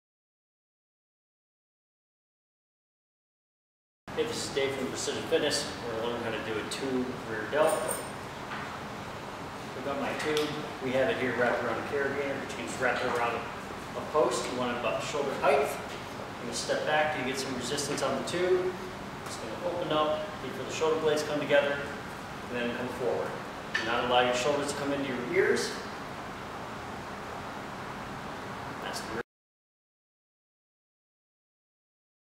Precision Fitness Tube Rear Delt
The tube rear delt can be a great way to build rear delt strength.